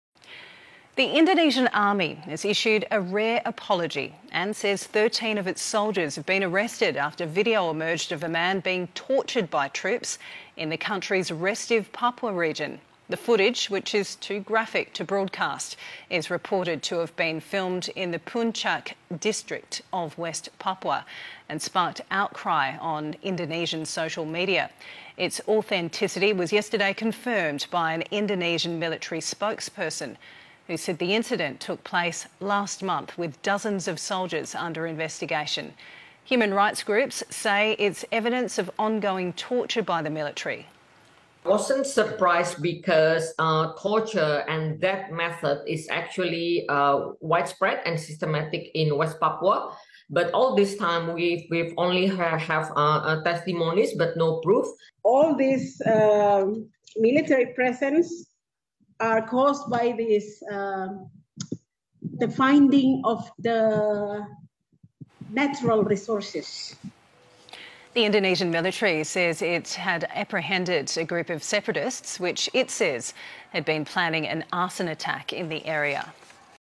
Stasiun televisi Australia Tayangkan Kasus Penyiksaan Warga sipil di Papua
Wamema, - Stasiun televisi Australia Tayangkan Kasus Penyiksaan Warga sipil di Papua.

Semoga Terungkap kasusnya..!! Waa..